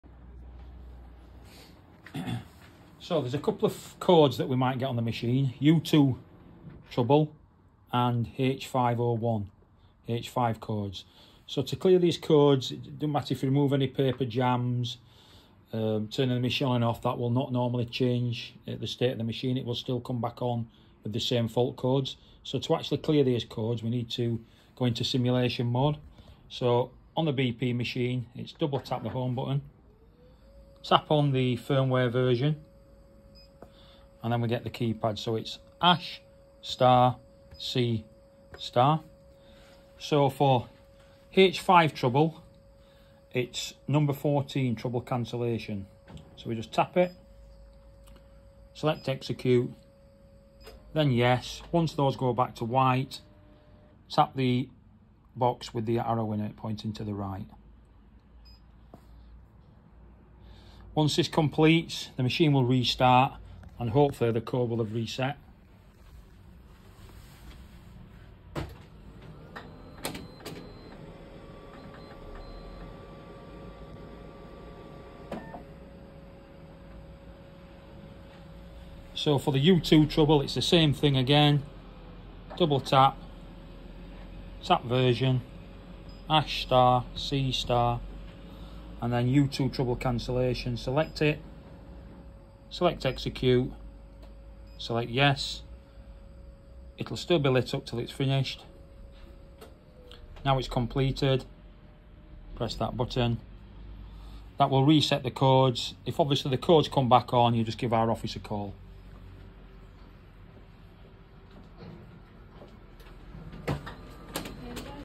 Sharp BP Range U2 and H5 codes
Steve from the Red Business Machines Team shows you how to troubleshoot the U2 and H5  error code on the Sharp BP range of Multi-Function printers.
See more of our how to videos on redmachines.co.uk/how-to-videos